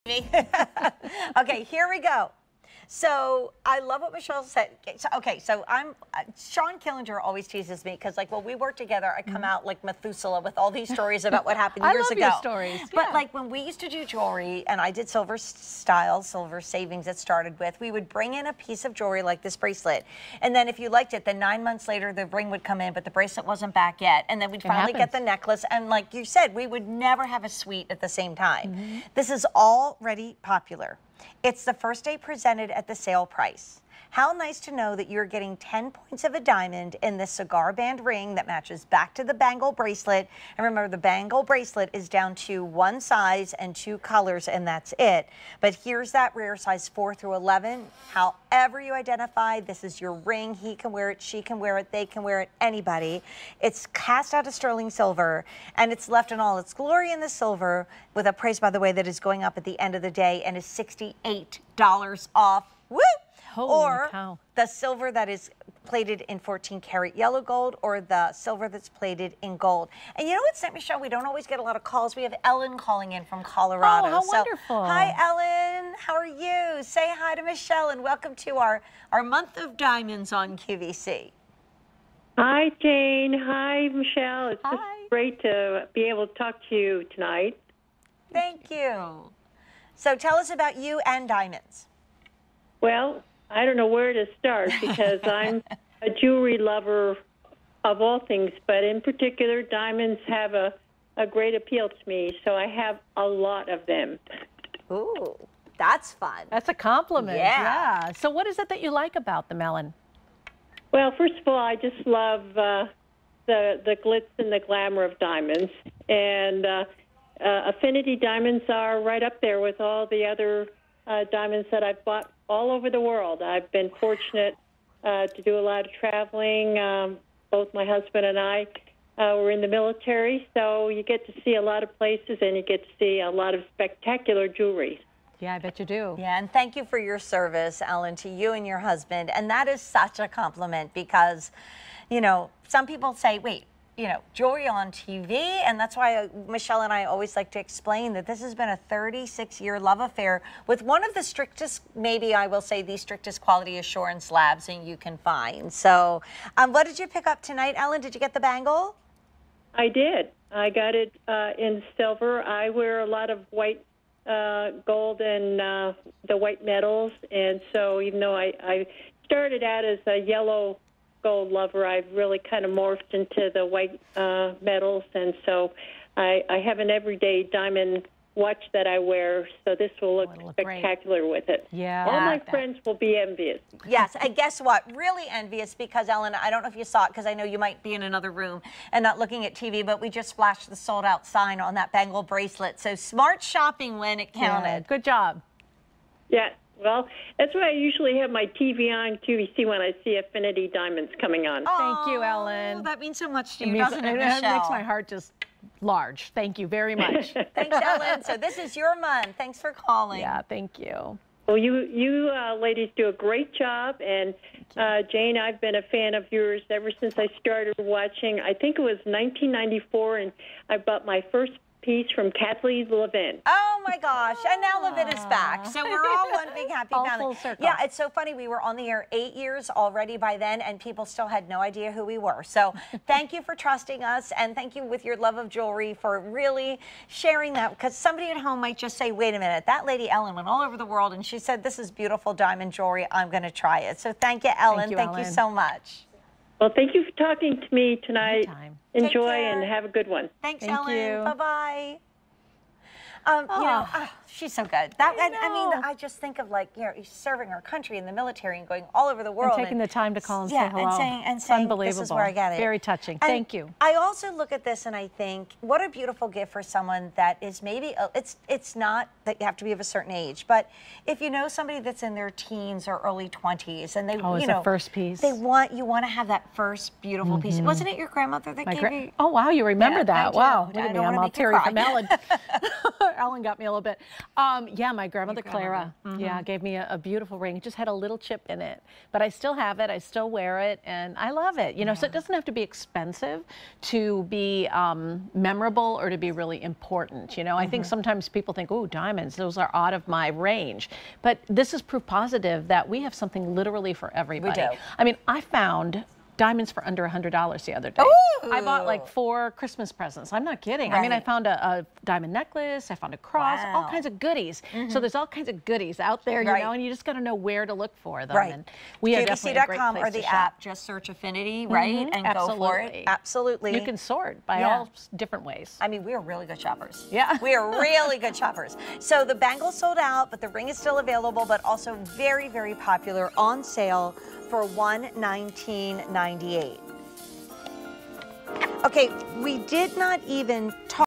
Affinity Diamonds Cigar Band Ring, Sterling 0.10cttw on QVC
Affinity Diamonds Cigar Band Ring, Sterling 0.10cttw

It may be a cigar band ring, but it's not just for sipping cognac in leather-wrapped chairs. This diamond-accented ring wants to be seen e-v-e-r-y-w-h-e-r-e. From Affinity&reg; Diamond Jewelry.